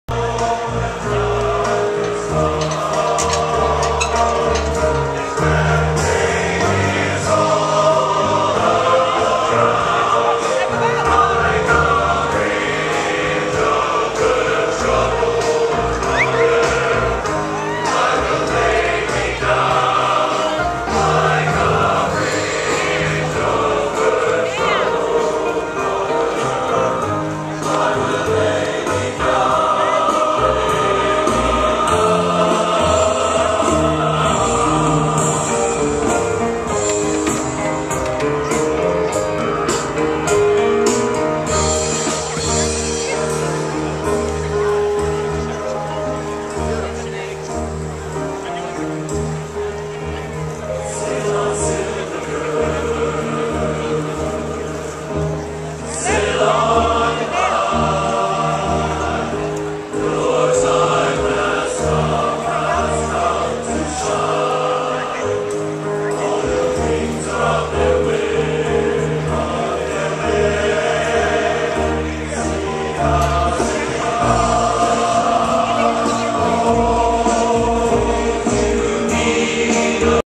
DC Gay Men's Chorus Opening At the Rally
DC Gay Men's Chorus provided great pre-rally entertainment at the National Equality March Rally.